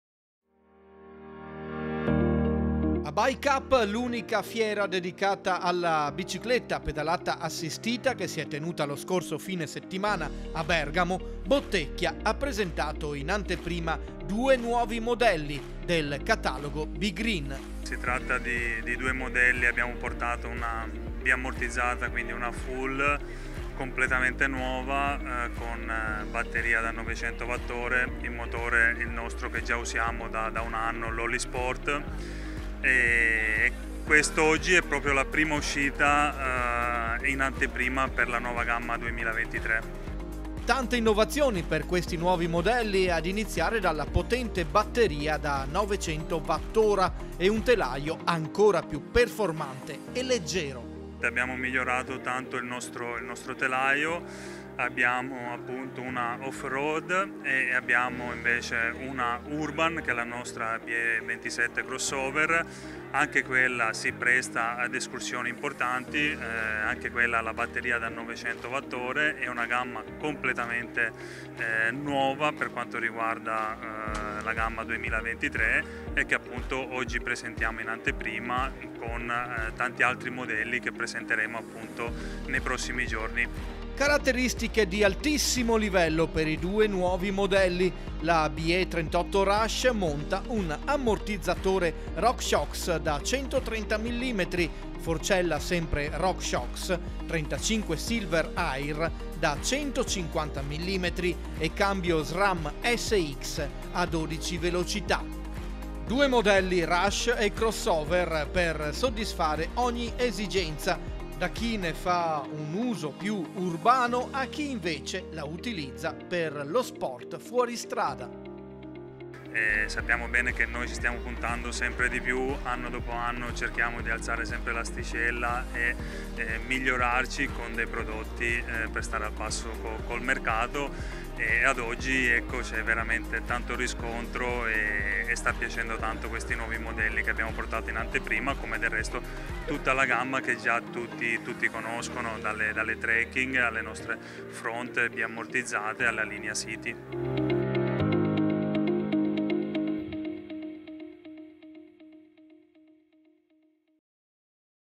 BOTTECCHIA A BIKEUP
Bottechhia presenta a Bike Up di Bergamo due interessanti novità 2023 in tema e-bike